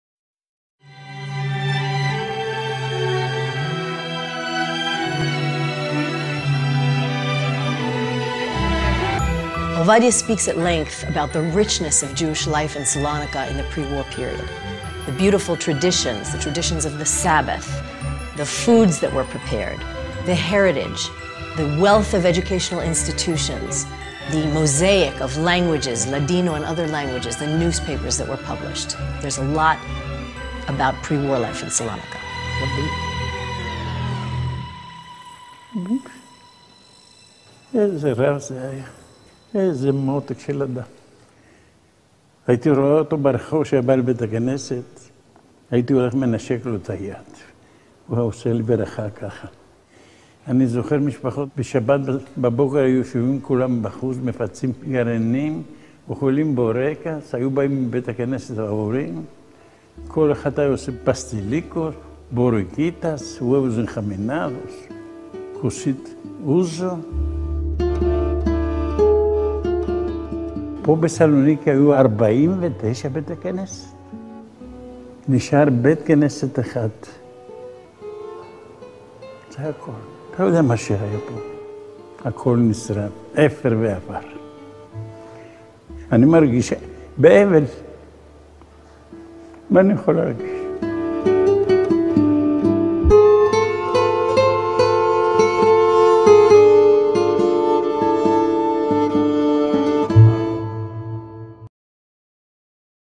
Jüdisches Leben in Salonika
Zeitzeugnisse stehen im Zentrum der Holocaust-Pädagogik, da sie die Geschichten von Individuen präsentieren. Mit der Zeit wird die unersätzliche Erfahrung, Überlebende aus erster Hand zu hören, und mit ihr die Empathie and emotionale Verbindung, nicht länger möglich sein. In diesem Video präsentiert Sheryl Silver-Ochayon den Film „Dir in Liebe gedenken“, in welchem der Überlebende Owadjah Baruch seine eigene Geschichte an den Orten erzählt, an denen die Geschehnisse stattfanden. Die Erfahrungen von Baruch, einem griechischen Juden, der in Salonika geboren wurde, erlauben einen Blick in die oft übersehene Geschichte des Holocaust der griechischen Juden. Der Film ist Teil der Reihe biographischer Dokumentarfilme über Zeitzeugen.

Sheryl Silver-Ochayon ist Mitarbeiterin der Internationalen Schule für Holocaust-Studien in Yad Vashem.

1. Teil: Die Verwendung von Zeitzeugenfilmen im Unterricht
2. Teil: Jüdisches Leben in Salonika
3. Teil: Die Deportation von griechischen Juden nach Auschwitz
4. Teil: Der Prozess der Entmenschlichung in Auschwitz
5. Teil: Tägliches Leben in den Lagern
6. Teil: Überleben und Rückkehr ins Leben